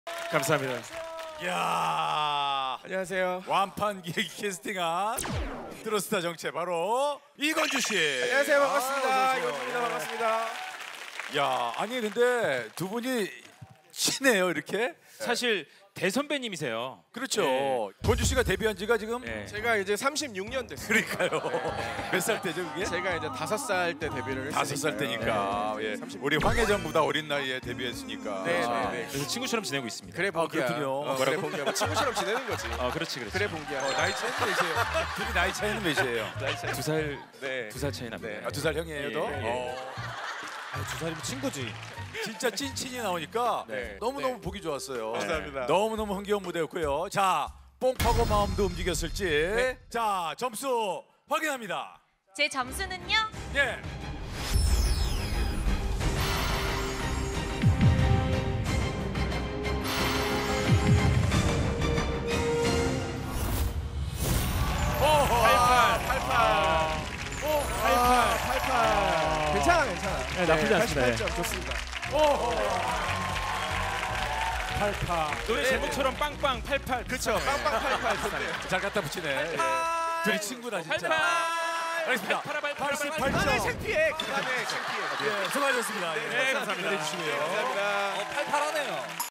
'순돌이' 이건주, 2살 형에게 반말하는 사연?! MBN 210210 방송
'순돌이' 이건주, 2살 형에게 반말하는 사연?!